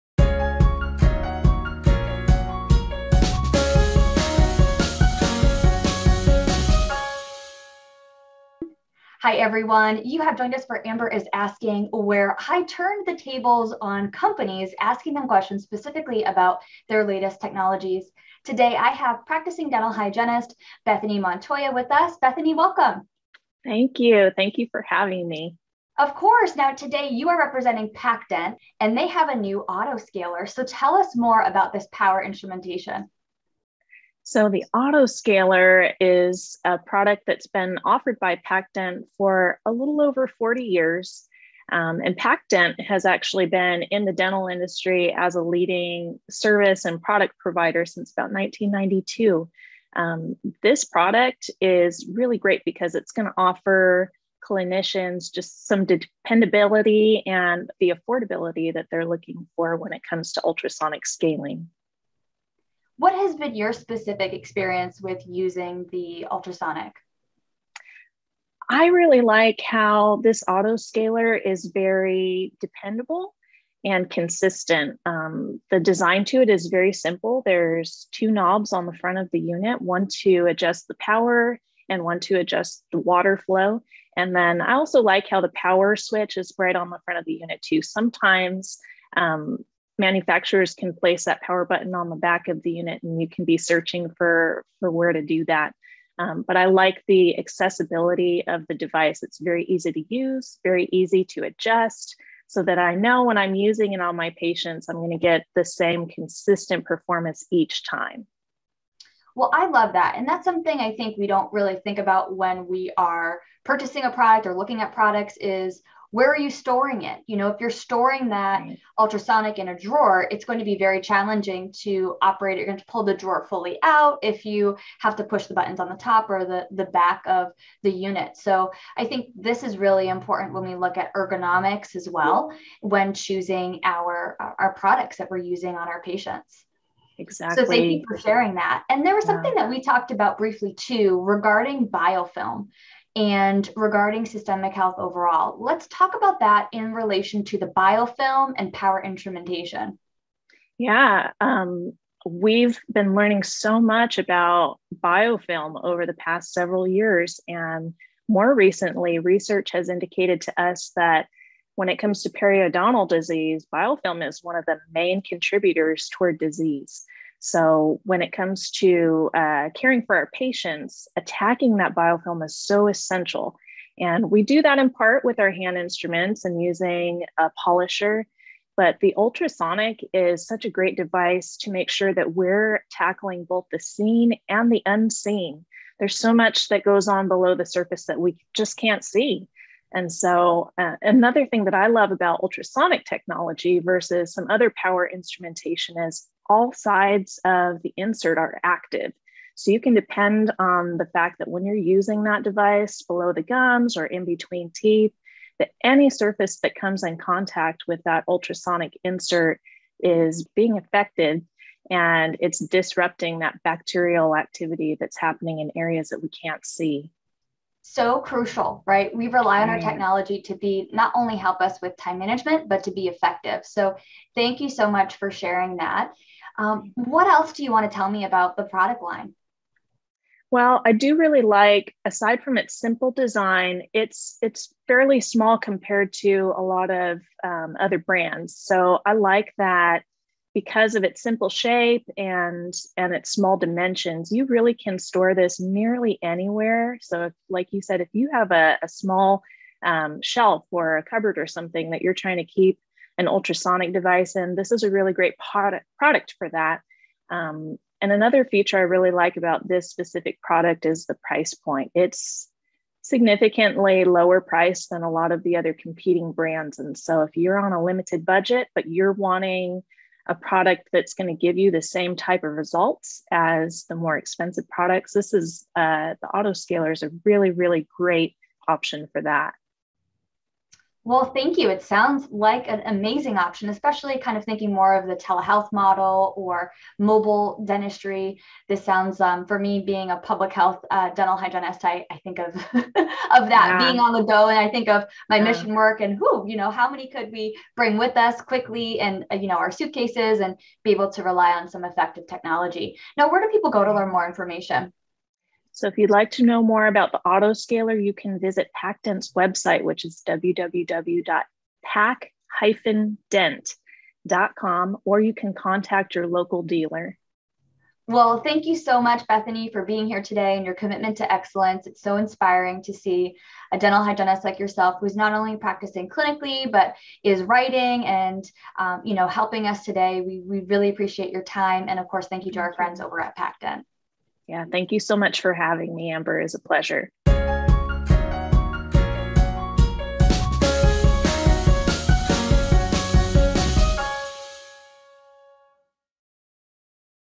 Amber Is Asking: Why the Pac-Dent AutoScaler® could be your perfect ultrasonic
There's more and more evidence that biofilm removal is at the center of good oral health, but you may not think you can afford or store a quality ultrasonic scaler. If you don't have a lot of money or room, or you need something portable, the Pac-Dent AutoScaler® could meet all your needs. Find out why!